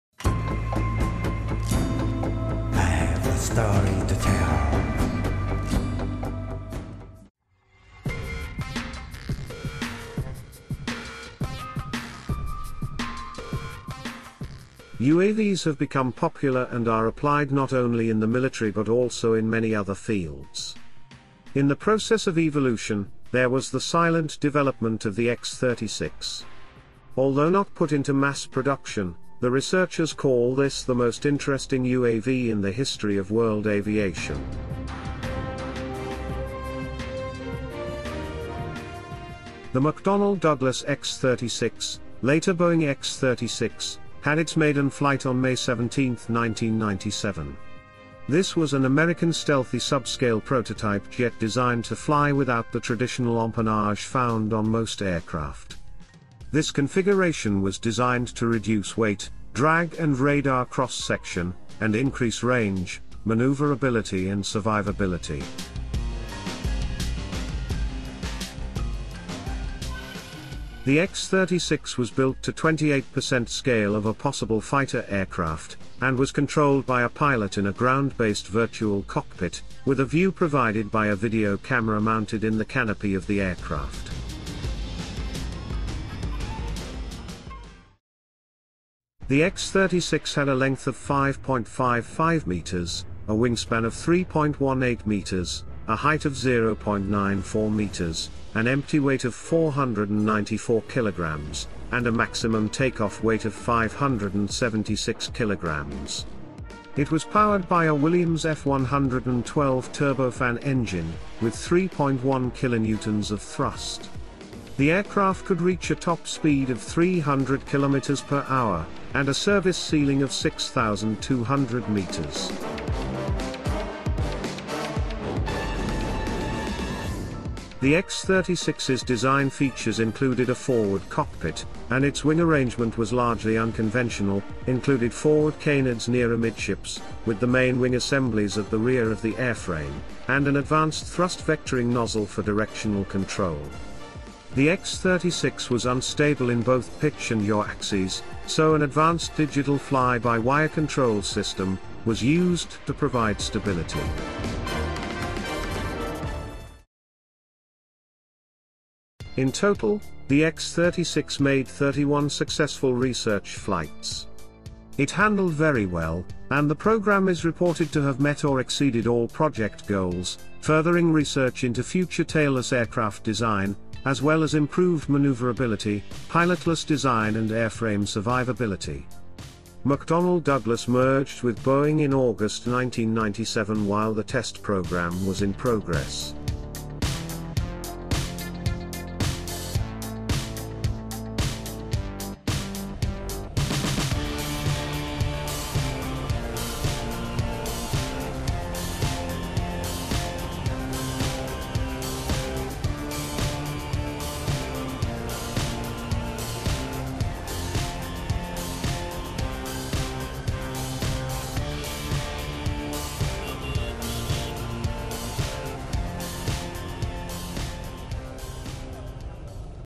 McDonnell Douglas X-36: Great Unmanned Fighter Technology Demonstrator In Aviation History
The McDonnell Douglas X-36, later Boeing X-36 had its maiden flight on May 17, 1997. This was an American stealthy subscale prototype jet designed to fly without the traditional empennage found on most aircraft. This configuration was designed to reduce weight, drag and radar cross section, and increase range, maneuverability and survivability.